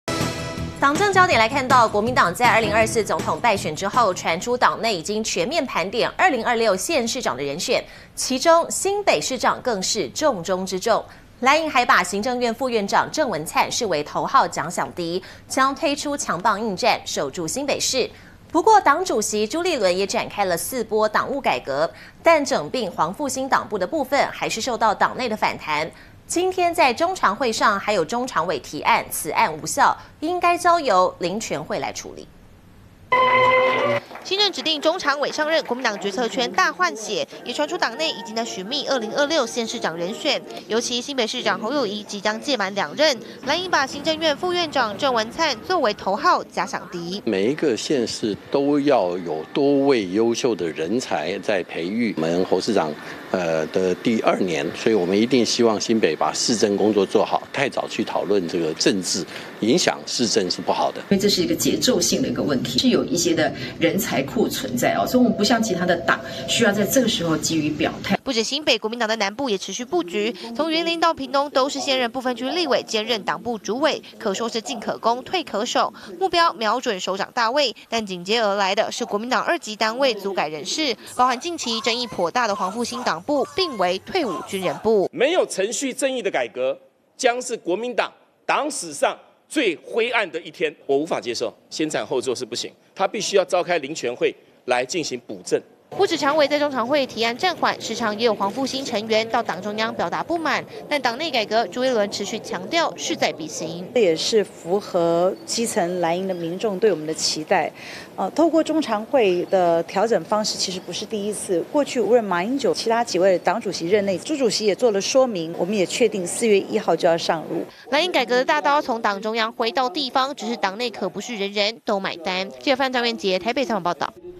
瞄準南部首長大位 不分區立委兼任黨部主委
國民黨 在2024總統敗選後，傳出黨內已經全面盤點2026縣市長的人選，其中新北市長，更是重中之重，藍營還把行政院副院長鄭文燦，視為頭號假想敵，將推出強棒應戰，守住新北市。不過，黨主席朱立倫，也展開4波黨務改革，但整併黃復興黨部的部分，還是受到黨內反彈，今天(20日)中常會上，還有中常委提案，此案無效，應該交由「臨全會」來處理。